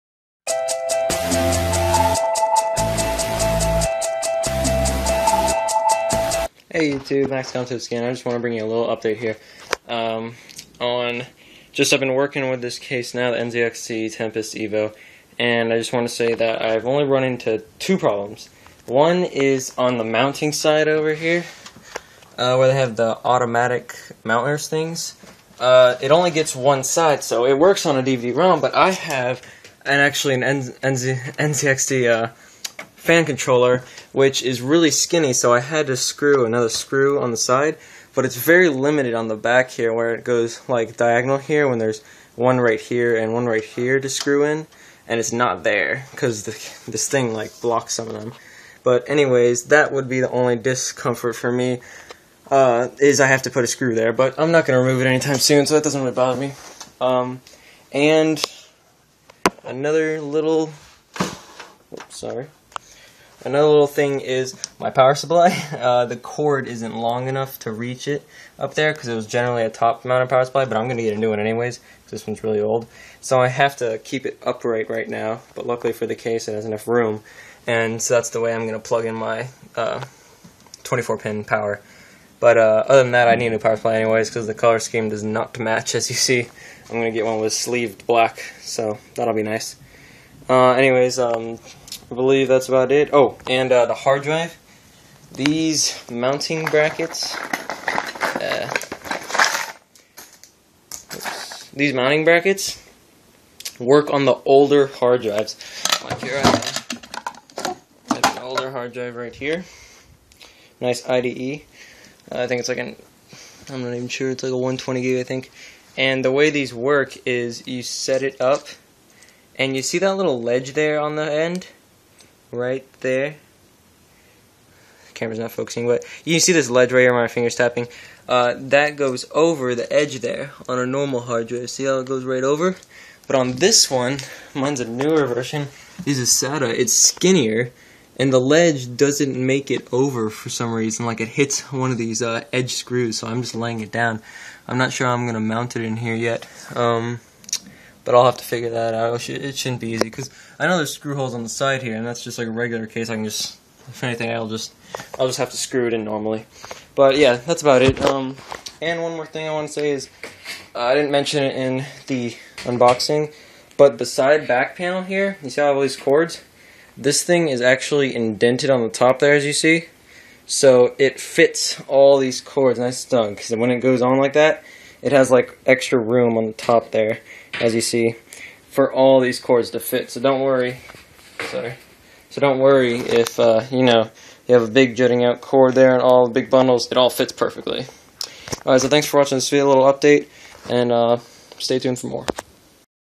NZXT Tempest Evo Update
Just a quick update on my build in the NZXT Tempest Evo.